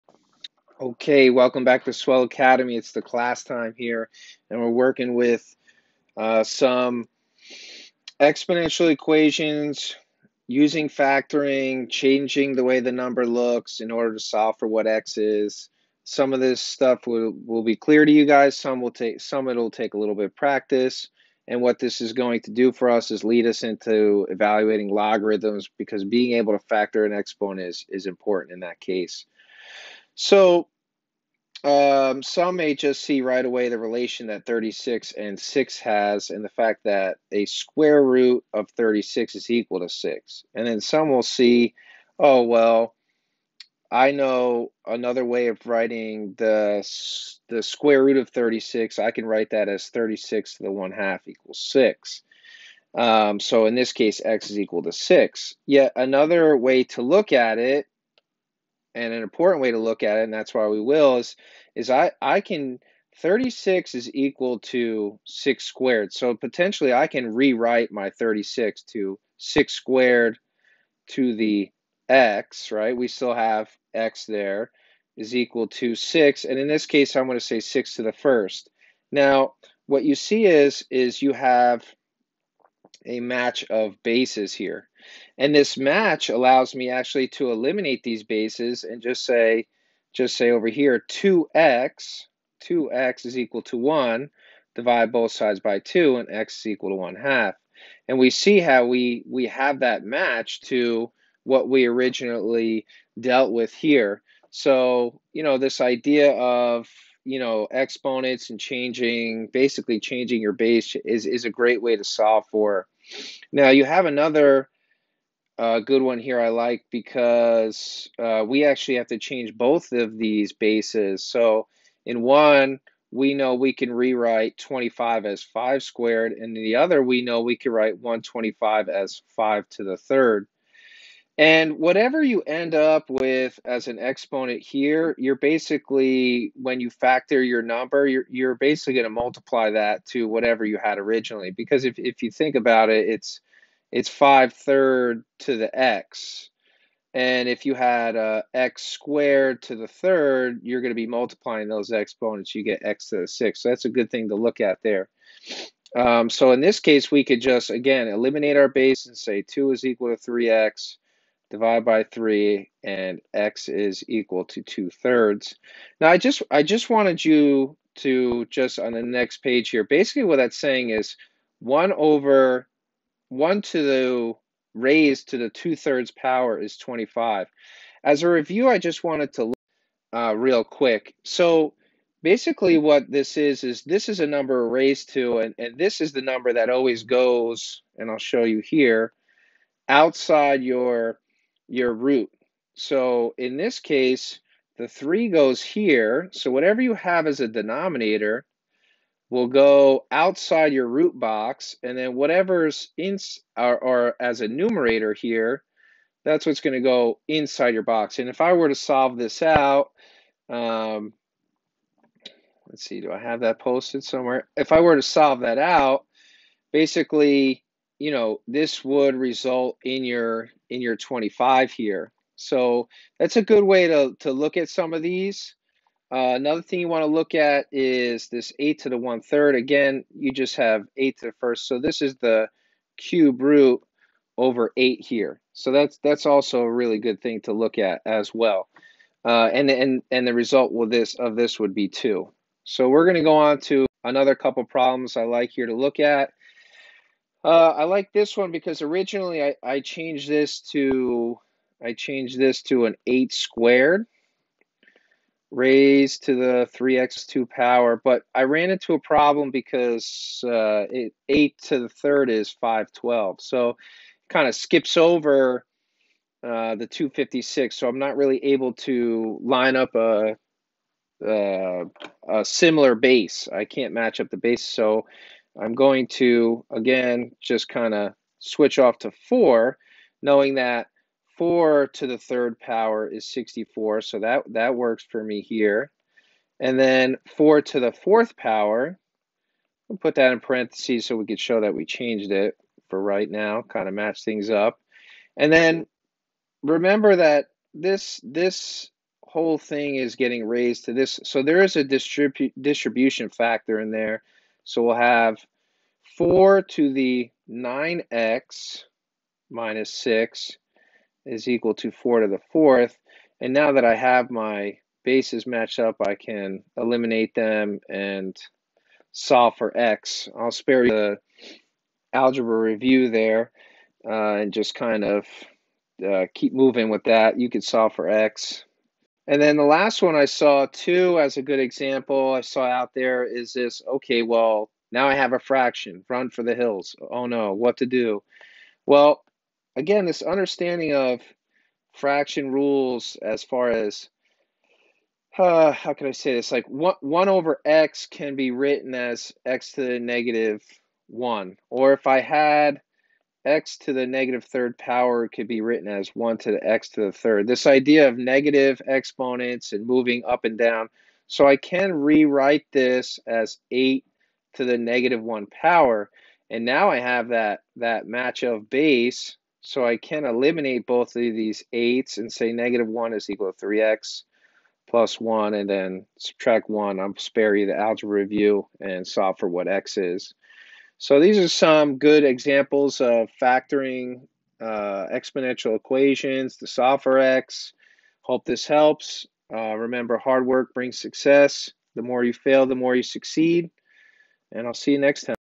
Solve Exponential Equations using Factoring
Learn how to solve exponential equations by factoring to match up your base numbers an solve for x